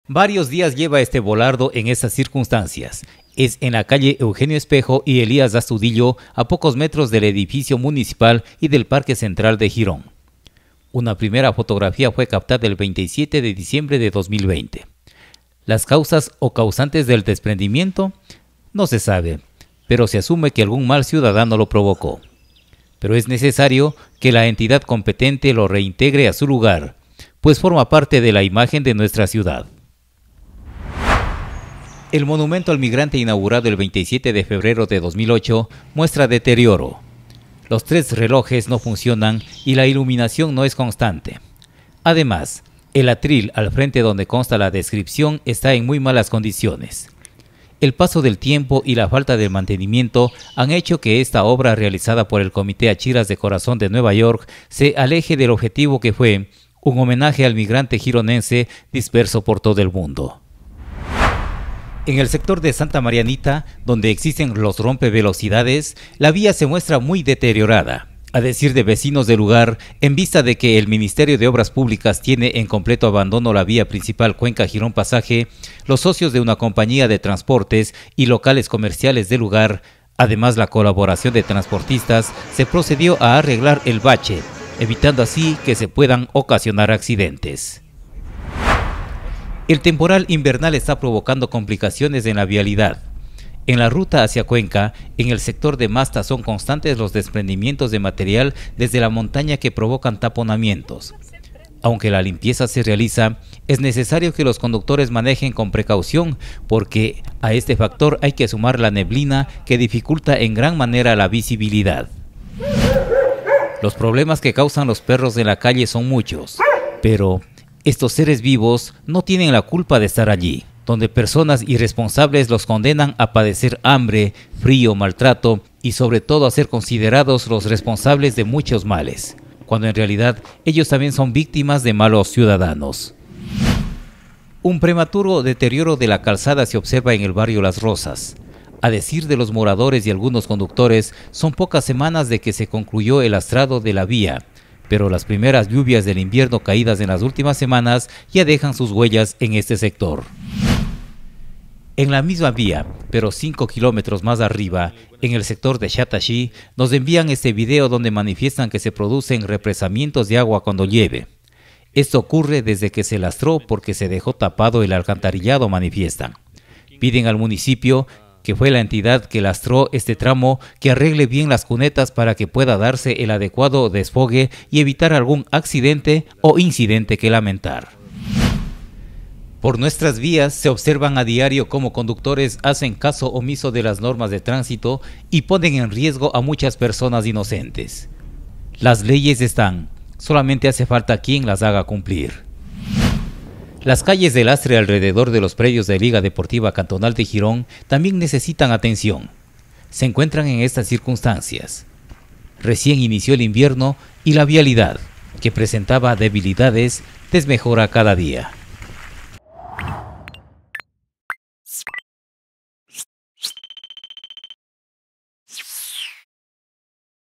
Cortos informativos
Caminando por las calles descubrimos detalles de la ciudad que necesitan atención; de la misma forma recorriendo las carreteras se avizoran varias falencias. Gracias a los amigos que nos envían sus vídeos y fotografías.